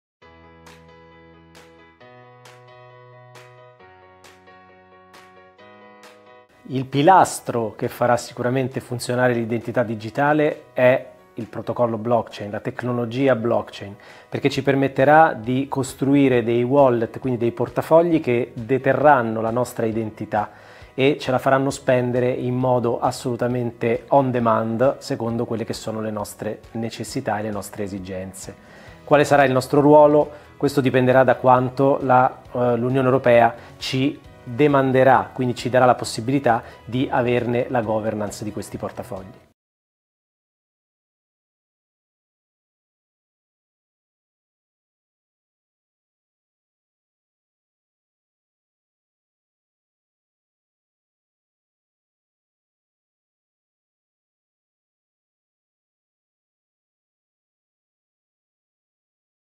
Identità digitale: il ruolo di cittadini e tecnologie - Alessandro Ghiani
Quale sarà il ruolo dei cittadini e delle tecnologie nell'evoluzione dell'identità digitale? Ne abbiamo parlato con Alessandro Ghiani al Money Pay Day 2023, l'evento organizzato da Money.it all'interno della Rome Future Week. Guarda il video completo